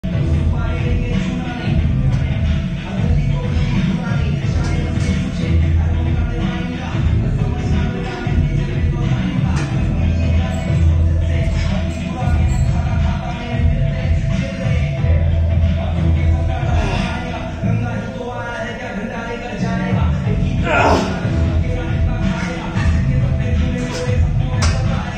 Overhead Press (52kg x 5)
Exercise: Overhead press
Weights:  52kgs
Reps: 5
Bodyweight: 77kgs

15 days after Covid Recovery
Day 6 (8.5 months gap)